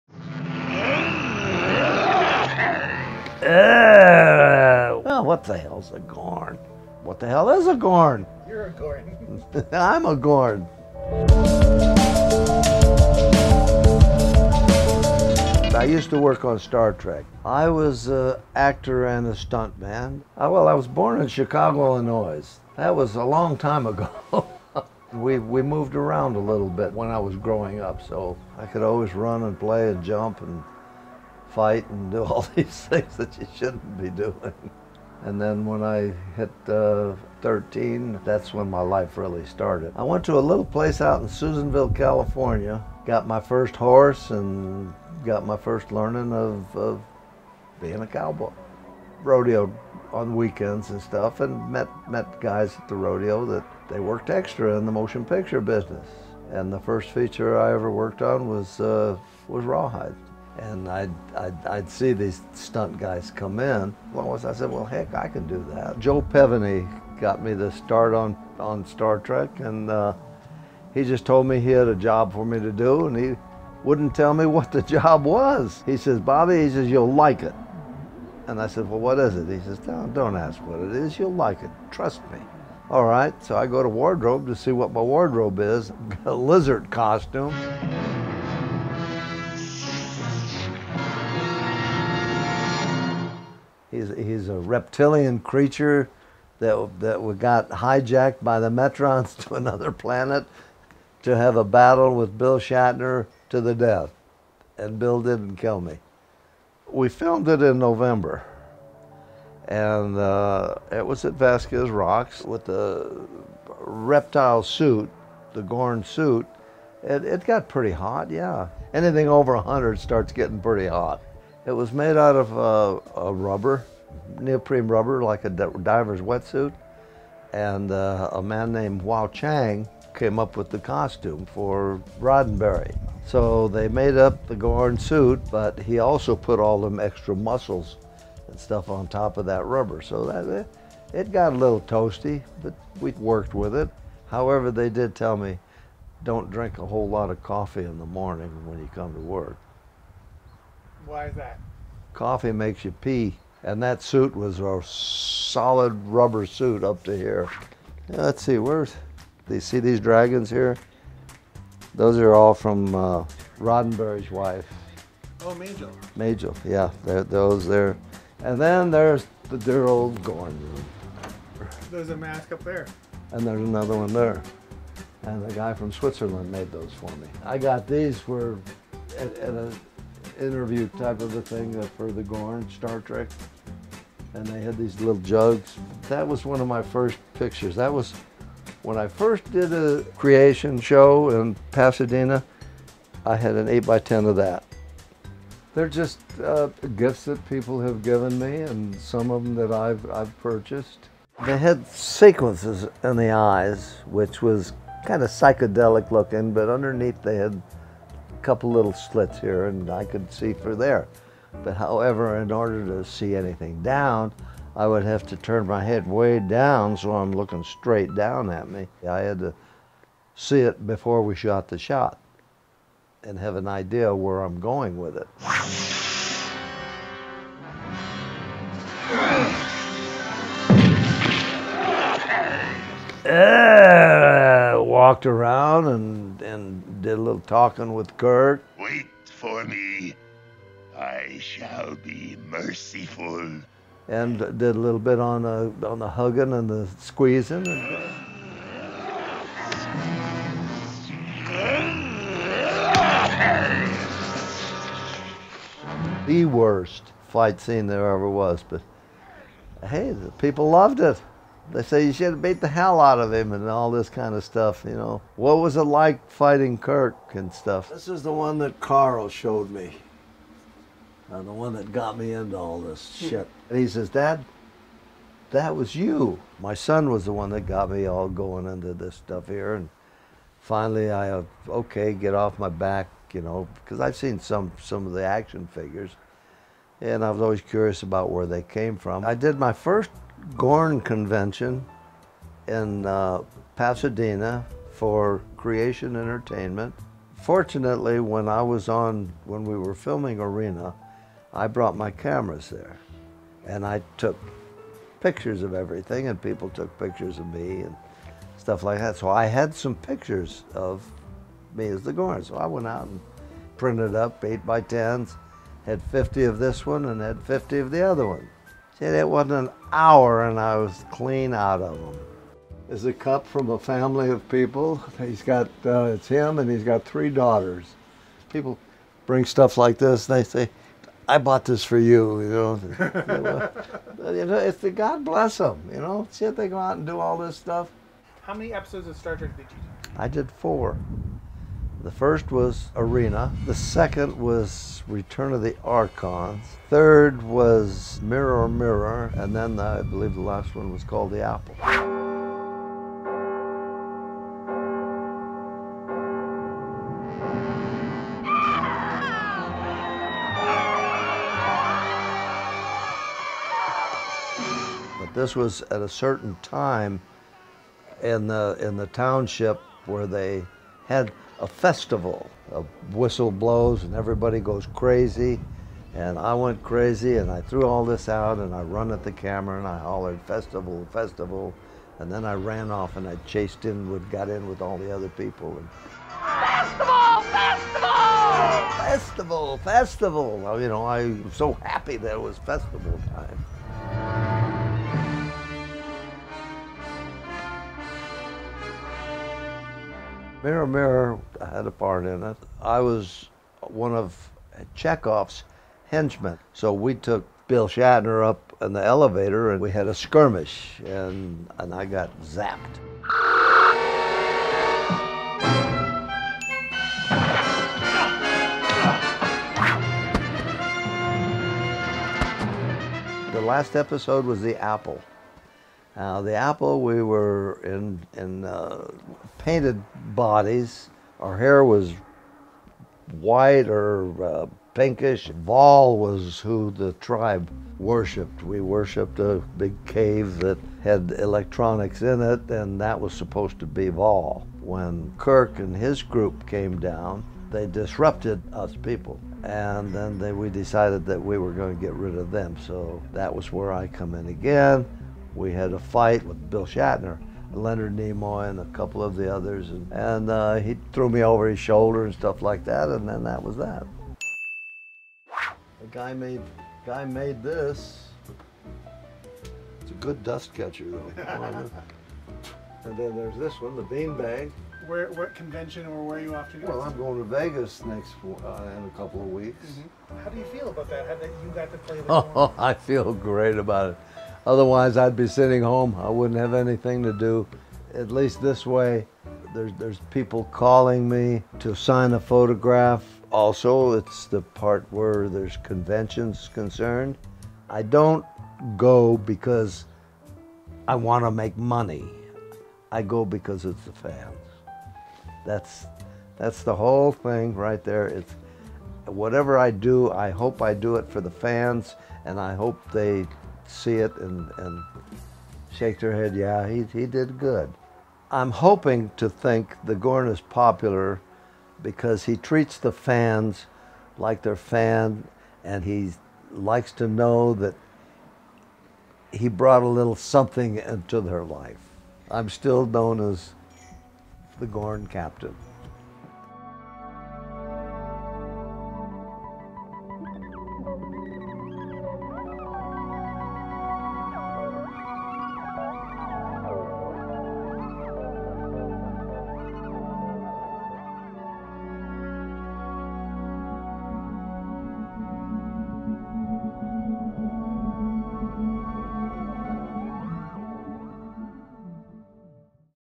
THE GORN | Interview with Bobby Clark
I recently had the pleasure to interview Hollywood stuntman Bobby Clark, known for playing THE GORN! Appearing in a single episode ("Arena" in 1966) the character is iconic and known to STAR TREK fans the world over. Bobby went on to play characters in three more Trek episodes: "Return of the Archons", "Mirror, Mirror" and "The Apple".

He's also performed as a stuntman in legendary cowboy series RAWHIDE, GUNSMOKE, and THE VIRGINIAN, as well as cult classics THE CAR and THE DRIVER. He even took part in zero-G tests for 2001: A SPACE ODYSSEY!

Now retired, Bobby talks about his career and how he came to be cast in Star Trek. I sat down with him at his ranch in Santa Clarita, not too far from Vasquez Rocks, where the Gorn episode was filmed.